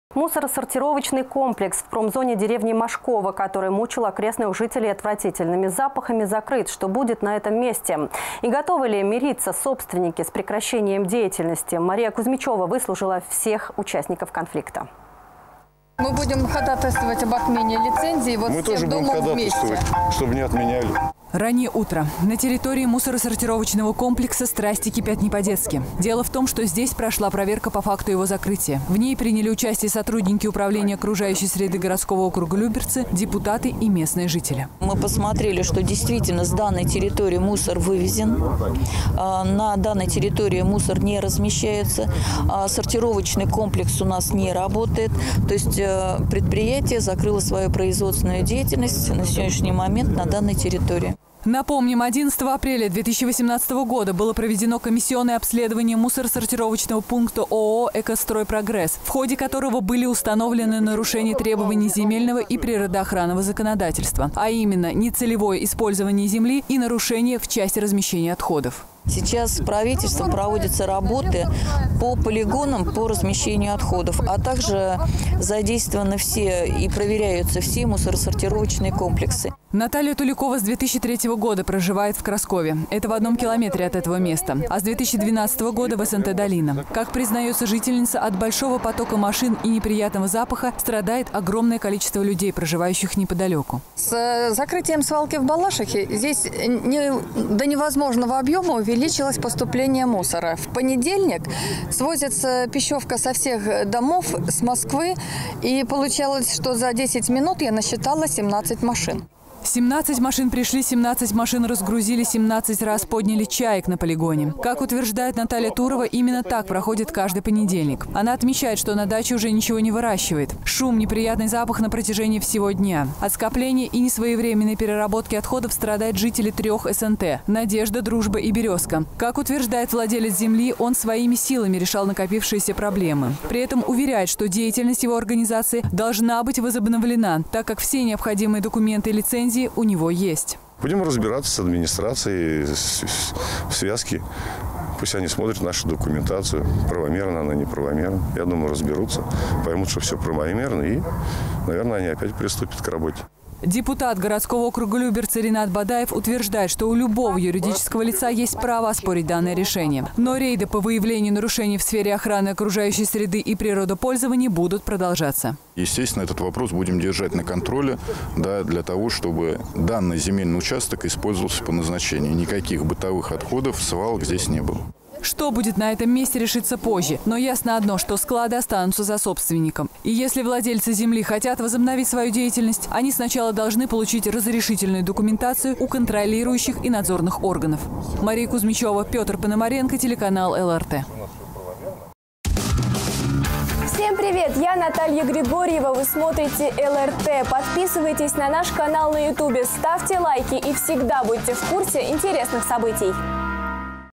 Работу мусоросортировочного комплекса приостановили в Люберцах
Мусоросортировочный комплекс в промзоне  деревни Машково, который мучил окрестных жителей отвратительными запахами, закрыли.  Что будет на этом месте и готовы  ли мириться собственники  с прекращением деятельности?  Смотрите  в нашем сюжете.

Раннее утро. На территории мусоросортировочного комплекса  страсти кипят не по-детски.  Дело в том, что здесь прошла проверка по факту его закрытия. В ней приняли участие сотрудники Управления окружающей среды г.о.Люберцы , депутаты  и местные жители .

Елена Романова, заместитель начальника Управления по охране окружающей среды администрации г.о.Люберцы:
"Мы посмотрели, что действительно с данной территории мусор вывезен, на данной территории мусор не размещается. Сортировочный комплекс у нас не работает. То есть предприятие закрыло свою производственную деятельность на сегодняшний момент на данной территории".

Напомним, 11 апреля 2018 года  было проведено комиссионное обследование  мусоросортировочного пункта  ООО «Экостройпрогресс», в ходе которого были установлены  нарушения требований земельного и природоохранного законодательства, а именно:  нецелевое использование земли и нарушение в части размещения отходов.

Елена Романова, заместитель начальника Управления по охране окружающей среды г.о.Люберцы:
" Как вы знаете, сейчас остро стоит эта проблема. Она поднята не только на территории Люберецкого, но и по всей Московской области. Сейчас правительством проводятся работы по полигонам, по размещению отходов. А также проверяются все мусоросортировочные комплексы".

Наталья Туликова с 2003 года  проживает  в Краскове, это  в одном километре  от этого места. А с 2012 года  в СНТ «Долина».  Как признается жительница, от большого потока машин и неприятного запаха страдает огромное количество людей, проживающих неподалёку.

Наталья  Туликова,  председатель садового товарищества «Долина»:
"С закрытием свалки в Балашихе здесь до невозможного объема увеличилось поступление мусора. В понедельник свозится пищевка со всех домов, с Москвы, и получается что я за 10 минут насчитала 17 машин".

17 машин пришли, 17 машин разгрузили, 17 раз подняли чаек на полигоне. Как утверждает Наталья Турова, именно так проходит каждый понедельник. Она отмечает, что на даче уже ничего не выращивает. Шум, неприятный запах на протяжении всего дня. От скопления и несвоевременной переработки отходов страдают жители 3-х  СНТ: «Надежда», «Дружба» и «Березка». Чтобы предотвратить эту проблему была создана петиция и собрано более 1000 подписей. Поддержать Наталью пришли и другие жители.

Как утверждает владелец земли, он своими силами решал накопившиеся проблемы. При этом  уверяет, что деятельность его организации должна быть возобновлена, так как все необходимые документы и лицензии у него есть.

Андрей Папичев, собственник земли:
"Будем разбираться с администрацией в связке, пусть они смотрят нашу документацию, правомерна она, неправомерна. Я думаю, разберутся. Поймут, что все правомерно, и,  наверное, они опять приступят к работе".

Депутат г.о.Люберцы  Ринат Бадаев, утверждает, что у любого юридического лица есть право оспорить данное решение. Но рейды по выявлению нарушений в сфере охраны окружающей среды и природопользования будут продолжаться.

Ринат Бадаев,  депутат г.о.Люберцы по 4 избирательному округу:
"Мы и дальше будем продолжать совместно с отделом окружающей среды г.о.Люберцы продолжать рейды на территории. И,естественно, этот вопрос будем держать на контроле для того, чтобы данный земельный участок использовался по назначению. Никаких бытовых отходов и свалок чтобы здесь не было".

Что будет на этом месте, решится позже. Но ясно одно, что склады  останутся за собственником. И если владельцы земли хотят возобновить свою деятельность, они сначала должны получить разрешительную документацию у контролирующих и надзорных органов.

Мария Кузмичева, Петр Пономаренко